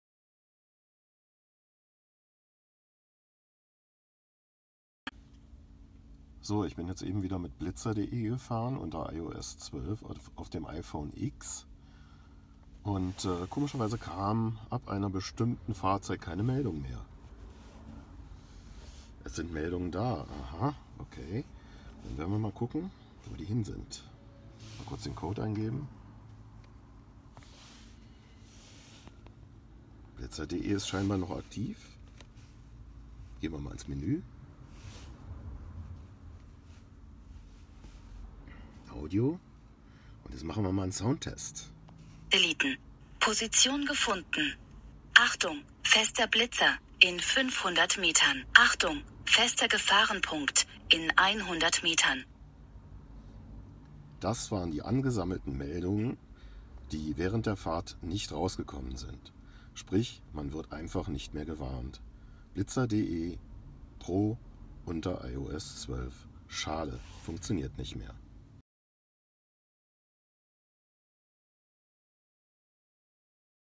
Blitzer.de PRO fehlende Warnungen Apple iPhone 7 & X
Auf Apple iPhone (zumindest mit iOS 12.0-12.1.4) warnt Blitzer.de PRO mit hoher Wahrscheinlichkeit nicht mehr akustisch, die Ansagen werden im Hintergrund "gesammelt" und können durch Auswahl von "Audiotest" dann gesammelt ausgegeben werden.
Weiterhin wird die App häufiger im Hintergrundbetrieb beendet, ohne dass der Nutzer davon etwas bemerkt. Auch dann wird man nicht mehr gewarnt.
App ist nicht zuverlässig nutzbar unter iOS!